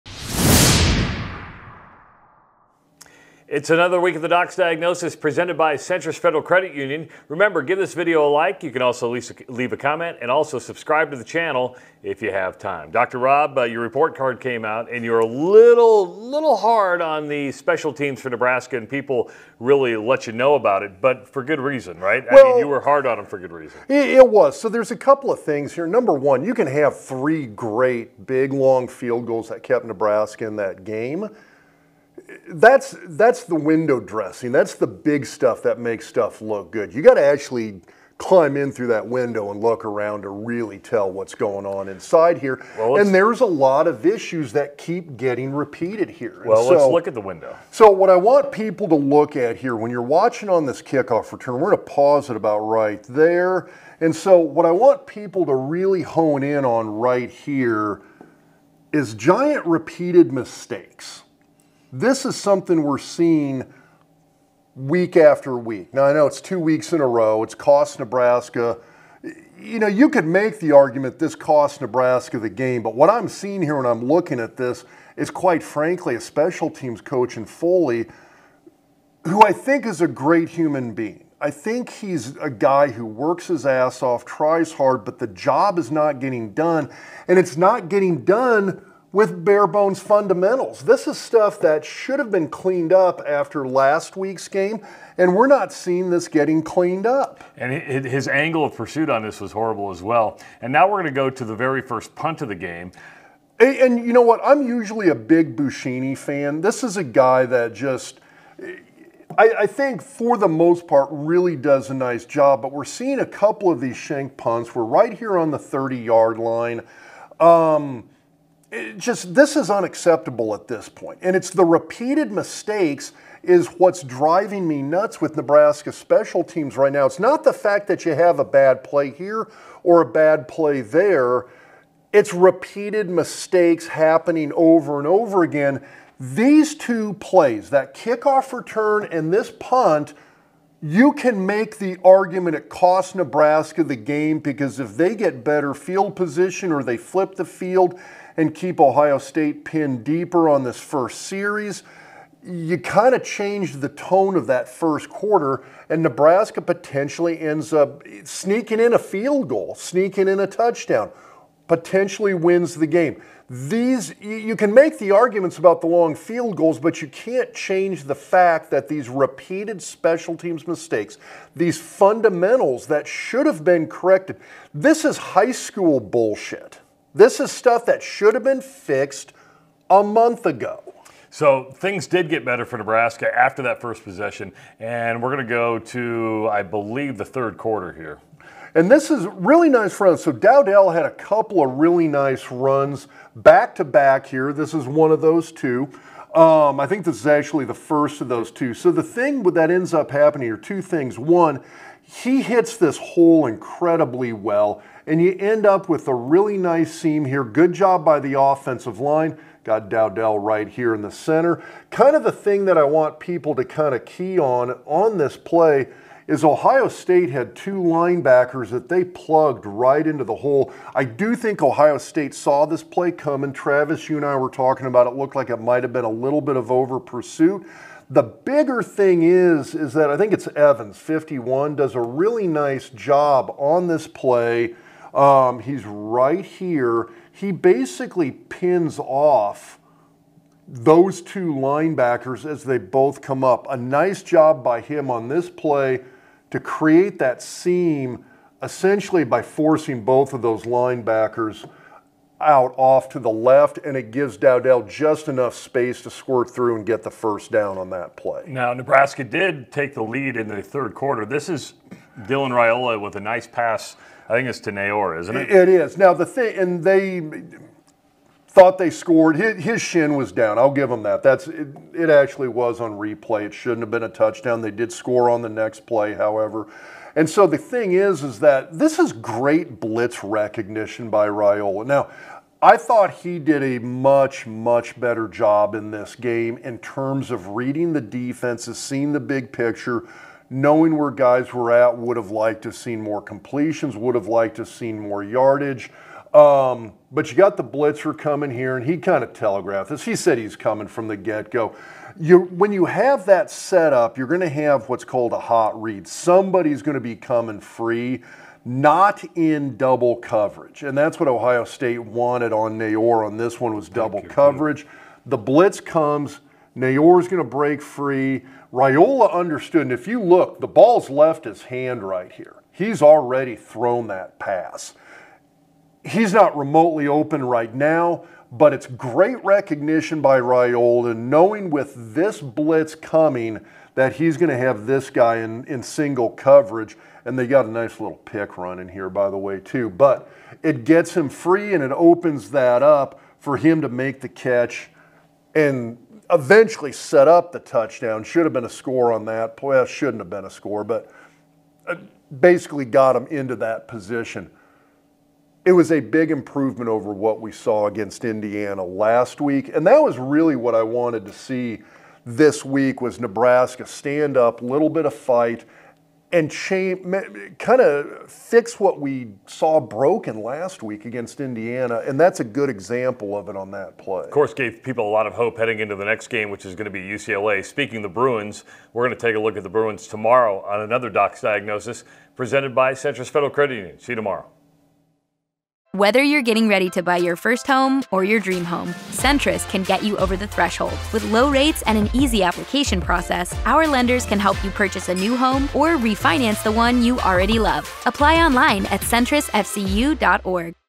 Doc's Diagnosis : Ohio State Game : The Special Teams NEED Work
Dr. Rob takes a look at some of the potentially game losing mistakes by the Husker's special teams in the game against Ohio State. Rob gets pretty fired up, and rightly so, the specials teams have been a consistent problem for the Huskers for several seasons now.